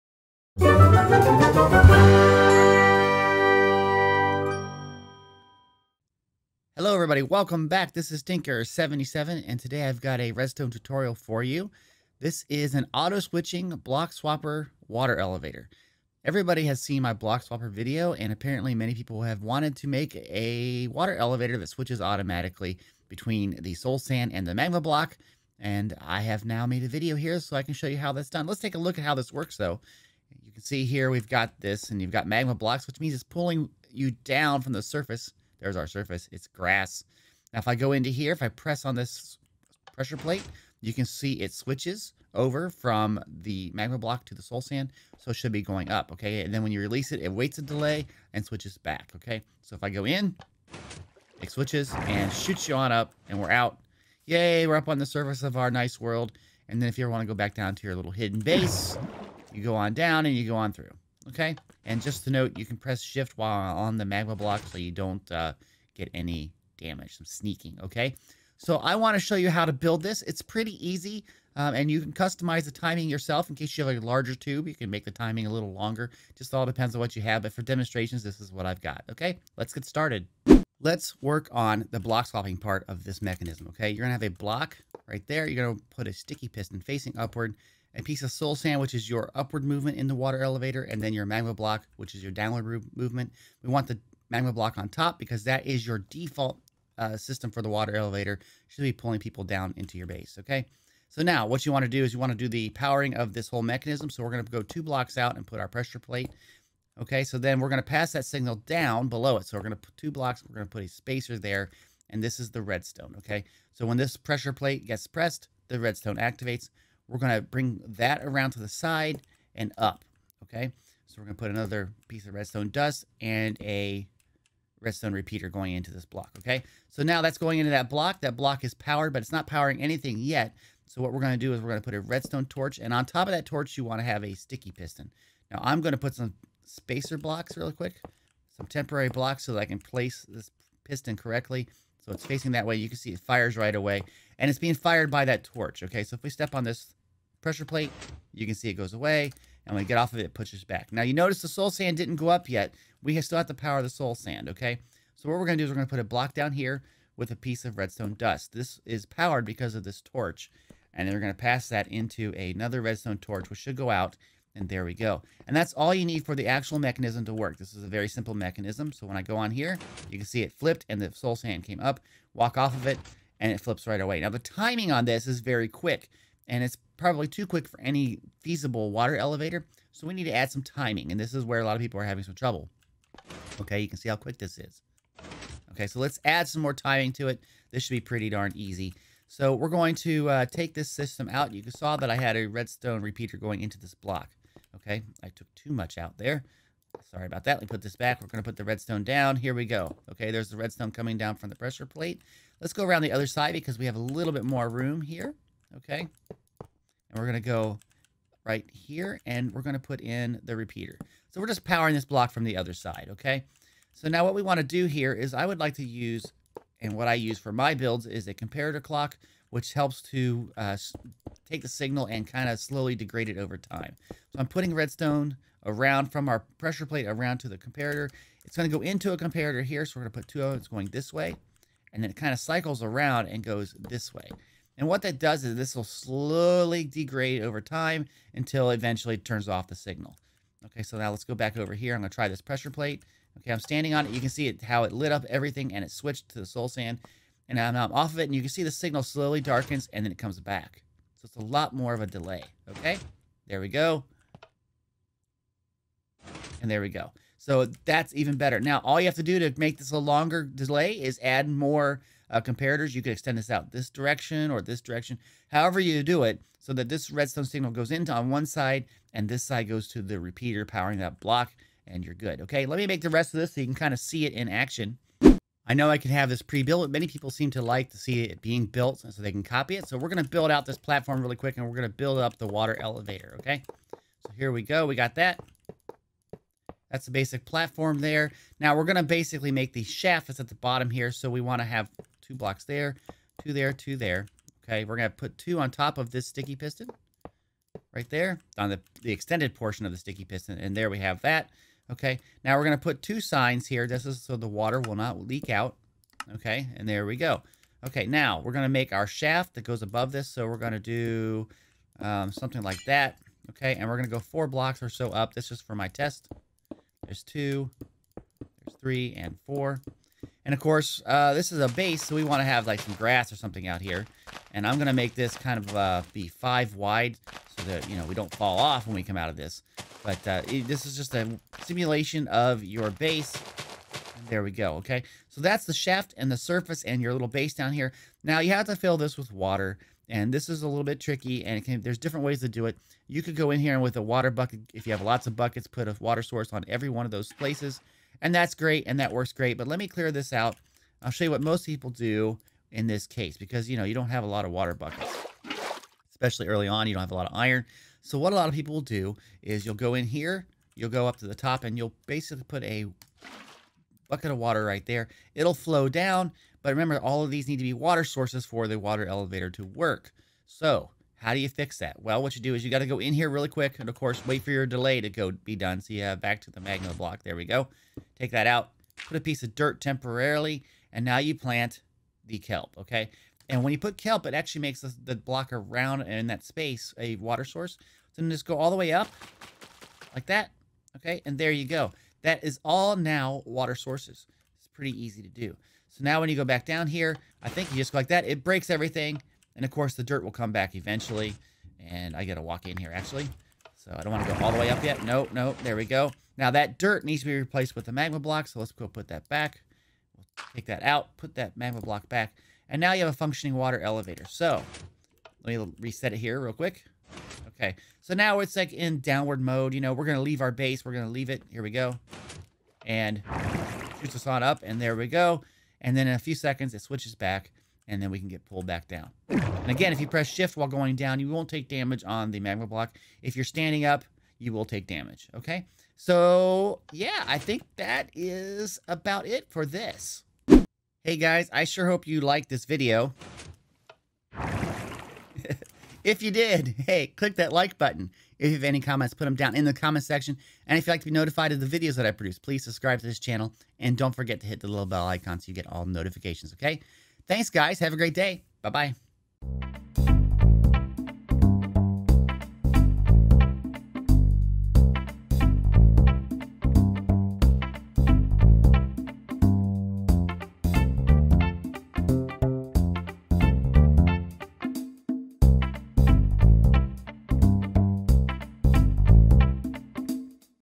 [1.15.2] - How To Build Auto Water Elevator! - YOU NEED THIS!
Hey everyone!  You've wanted this video for quite awhile!  This is an Automatic Water Elevator that uses a Block Swapper to switch between the Soulsand and the Magma Block!  Also included is an explanation of how to add a delay, so you can have a tall water elevator if you want!  Take a look and have fun building!